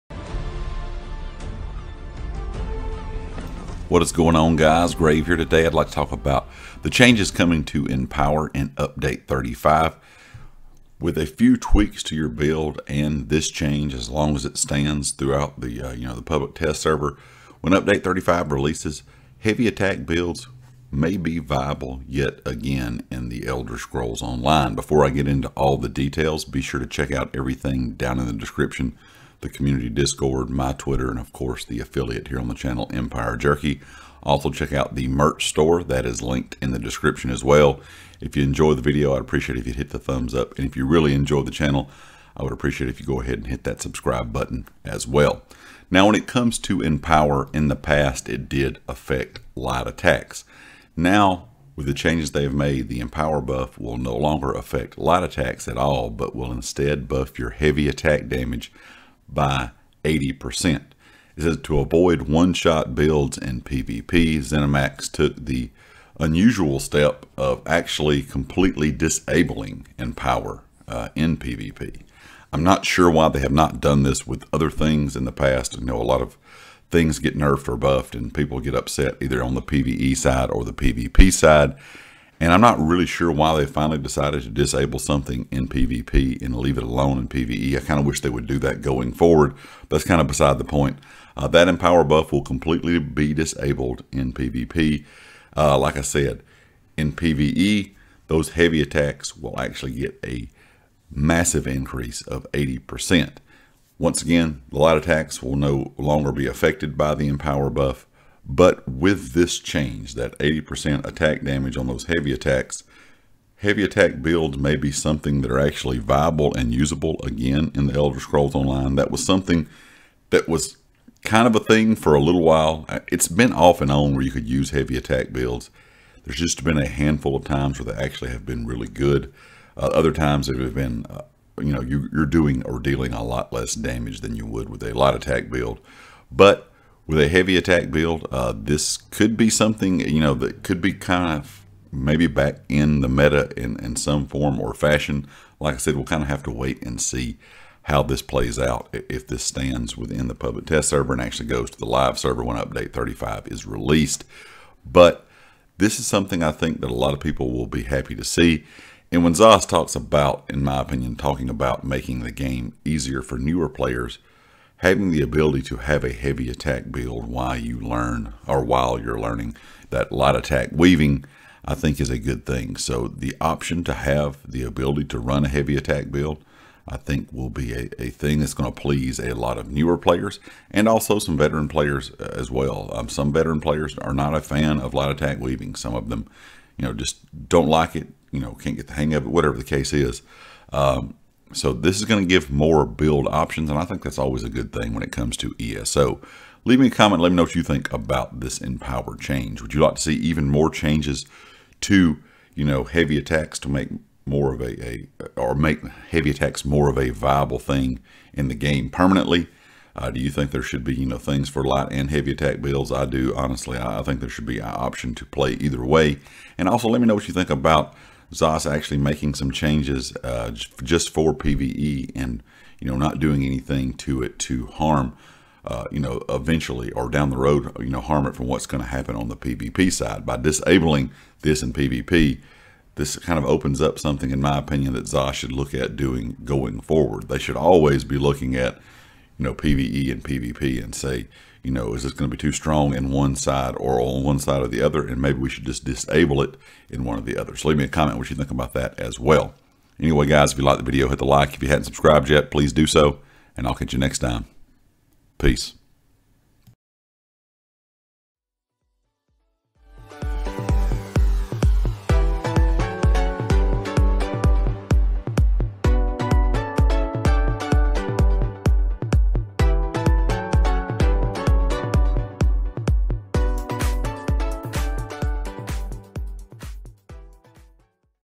🧙🏽ESO Update 35 Empower Changes | Heavy Attack Builds Are Back?
Update 35 will change how empower works and we may see the return of heavy attack buiklds because of this change.